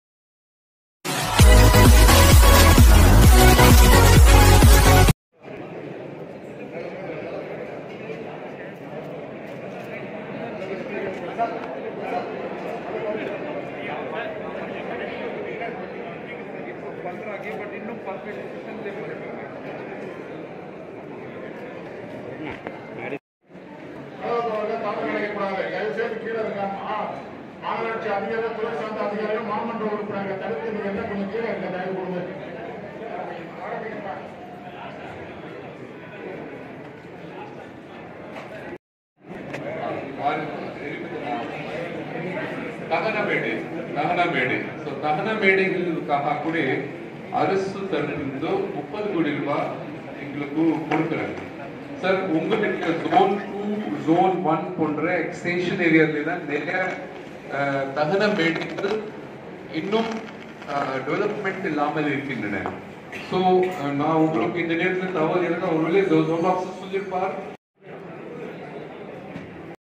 Review Meeting Projects of Manali zone (zone 2) will Presides by Hon'ble Mayor Priya & MC Arumugam
Review meeting Regarding projects of  manali  zone (zone 2) will presides by Hon'ble Mayor Priya & MLA TVT Shankar at manali zonal office Organisation MC Arumugam Kamarajar salai chennai- 600068